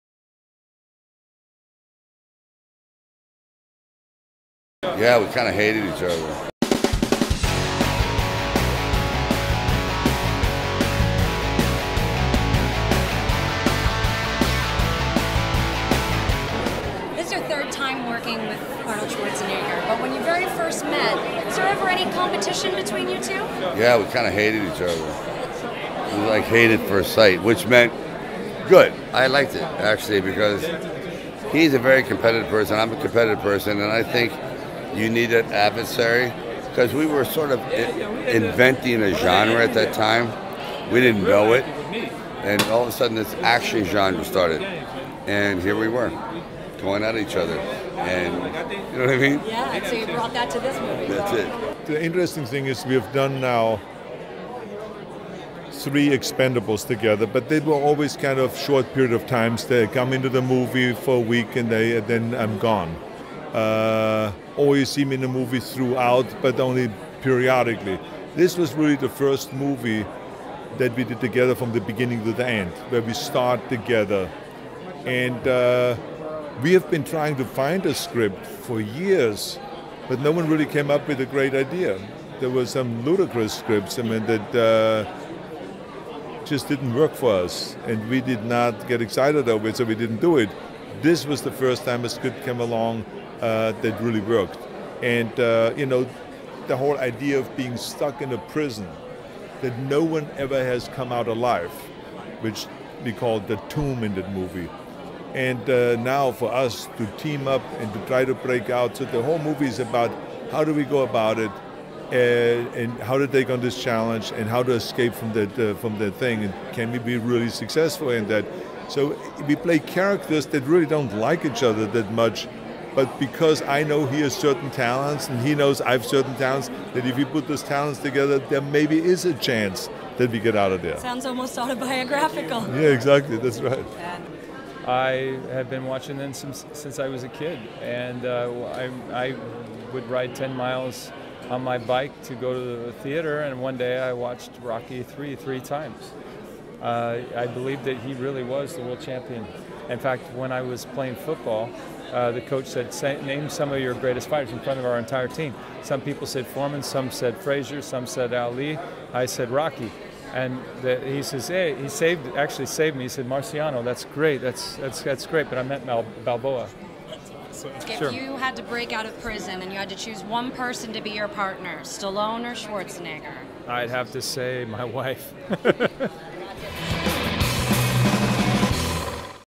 Arnold Schwarzenegger and Sylvester Stallone on the ESCAPE PLAN Red Carpet
Sylvester Stallone says when he first met Arnold Schwarzenegger in the 80s, it was "hate at first sight."

Two of the biggest names in the action genre joined forces as co-leads in ESCAPE PLAN and the two are telling CineMovie at the red carpet premiere in New York City why it took them years to find a project to work on.